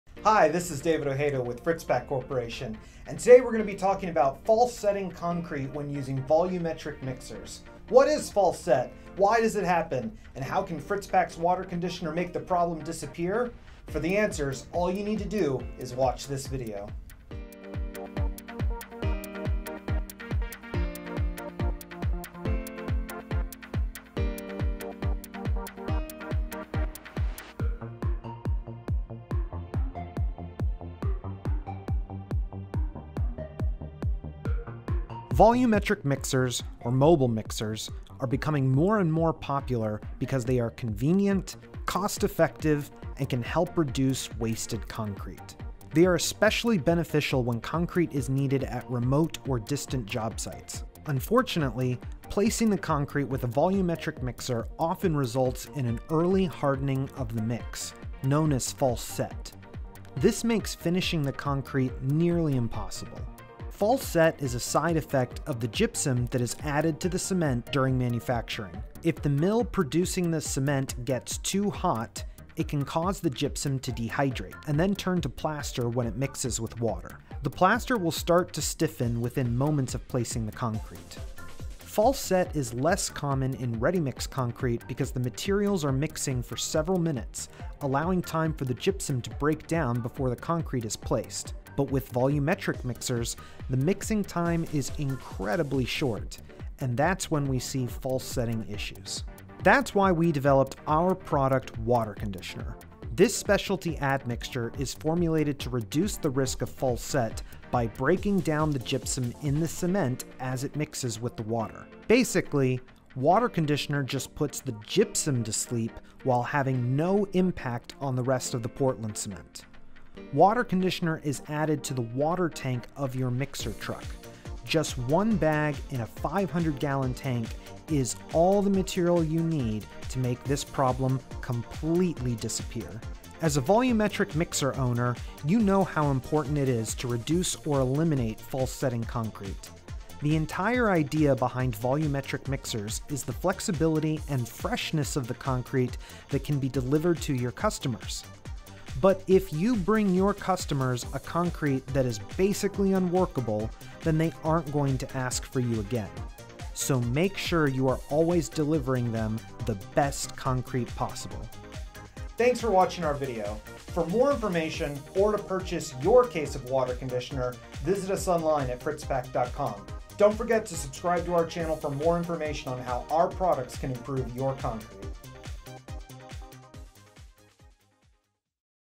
Prevent Concrete False Set with Fritz-Pak's Water Conditioner
00:00 Introduction
00:35 Benefits of Volumetric Concrete Trucks
00:51 What is False Set?
01:22 False Set and Volumetric Trucks
01:41 Fritz-Pak's Water Conditioner
02:13 Benefits for Volumetric Operators
02:42 Visit fritzpak.com today

Find out how using Fritz-Pak’s Water Conditioner can help eliminate the problem of concrete false set in your volumetric or mobile mix trucks.  This unique product is specially formulated to break down the gypsum in the cement that is at the root cause of false set problems.  In addition to learning about the benefits of our admixture, you’ll find out what causes false set to occur, why it doesn’t affect ready-mixed concrete as often, and some of the benefits of delivering concrete in volumetric trucks.
Shout out to Bay-Lynx & Brundage Bone Concrete Pumping for providing much of the footage used in this video.